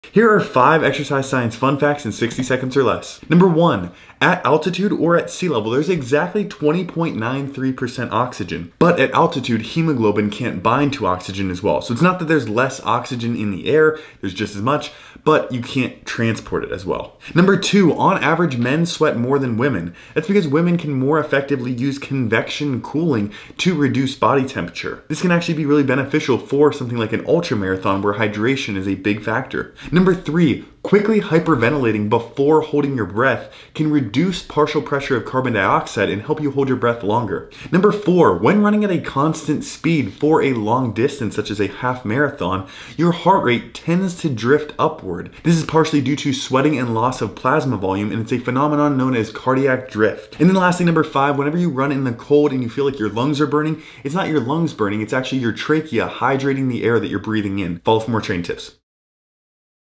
5 Surprising Exercise Science Fun Facts
Exercise Science Fun Facts🤓

1️⃣ At sea level and on a mountain there is exactly 20.93% oxygen in the air. So the air isn’t “thinner” but you can’t deliver oxygen to the muscles as well due to less hemoglobin binding. (Note: Technically as pressure drops from ~760mmHG at sea level to ~253-500mmHG at the peak of a mountain oxygen molecules are more spread out so it’s a combination of pressure change and oxygen delivery contribute. I have a full explanation on YouTube)

2️⃣ On average men sweat more than women. This is because women take better advantage of convection cooling, which can be an advantage in long competitions where hydration is a big factor.

3️⃣ Quickly hyperventilating for a few seconds before holding your breathe can decrease partial pressure of CO2 and help you hold your breathe longer.

4️⃣ During long endurance events, as you sweat you lose blood plasma volume leading to an increase in heart rate known as cardiac drift.

5️⃣ Your lungs don’t burn when you run in the cold, it’s actually your trachea hydrating the air.

Send this to someone who likes Exercise Science🔬